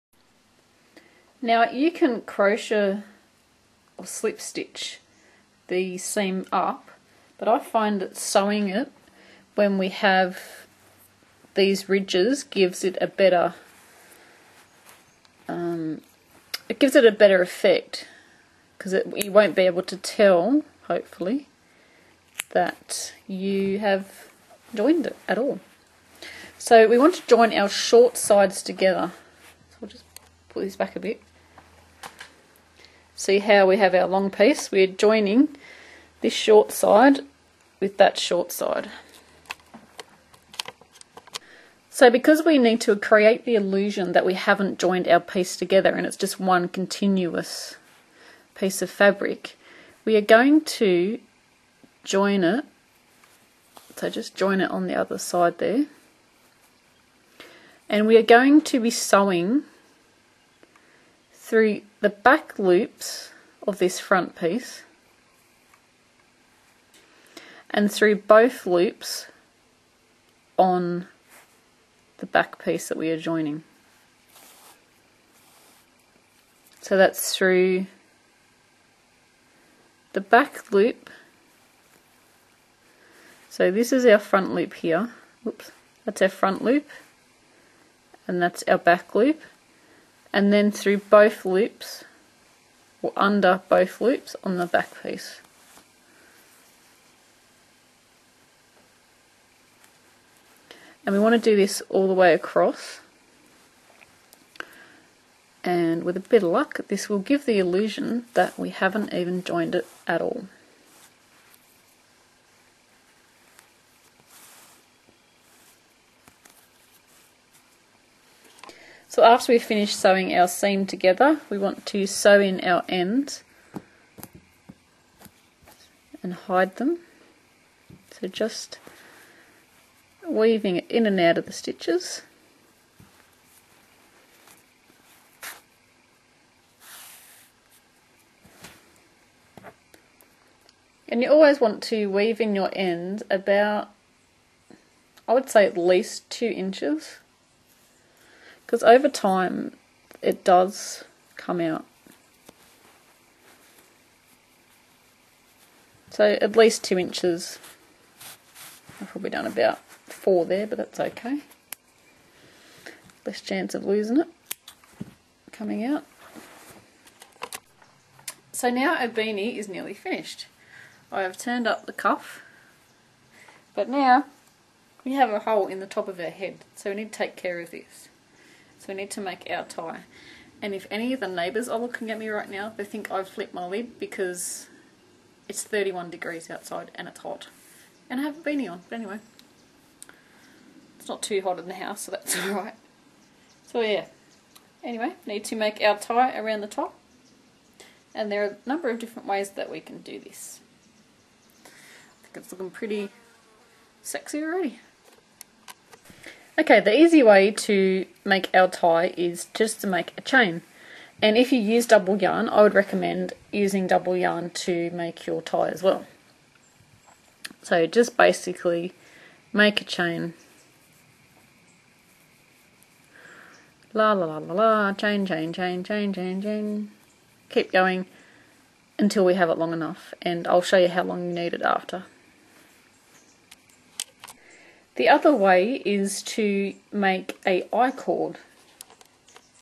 Crochet Beanie / Hat and Neck Warmer in 1 - Part 2 of 3 Tutorial
know as Bobwilson123, I rebranded in June 2021

The pattern that Beth uses is entitled, "The Convertible" by Deborah E Burger